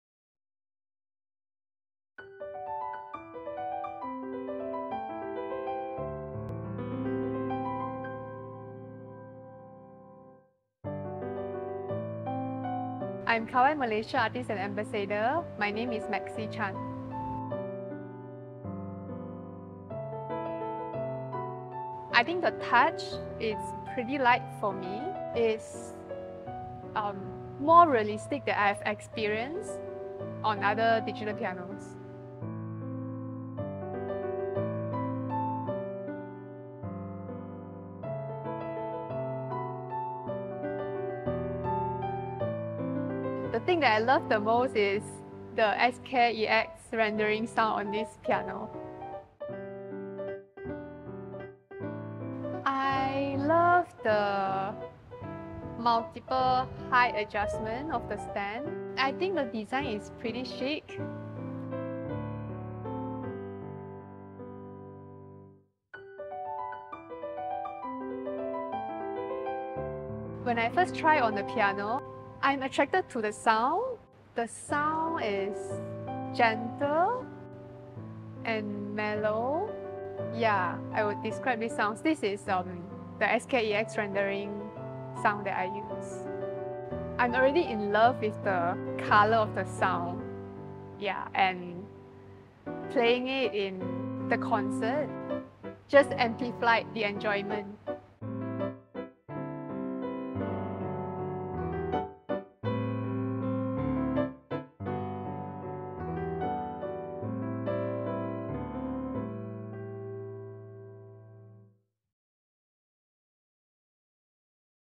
Kawai Hybrid Grand NV10S with Kawai Malaysia Ambassador Maxy Chan
Model: Kawai NV10S Hybrid Piano
■  Peerless Millennium III Hybrid grand piano keyboard action
■  Unique real grand piano damper mechanism
■   SK-EX Rendering piano sound engine with multi-channel sampling
■  Premium audio processing and amplification technologies
■  High-fidelity 7-speaker delivery system for enhanced depth
■  Integrated Bluetooth® MIDI and Audio wireless technology
■  Modern 5" LCD touchscreen display embedded within cheekblock
■  Flexible Virtual Technician function with 19 adjustable parameters
■  MP3/WAV playback, record, and overdub to USB
■  Stunning ebony polish cabinet with Soft Fall fallboard system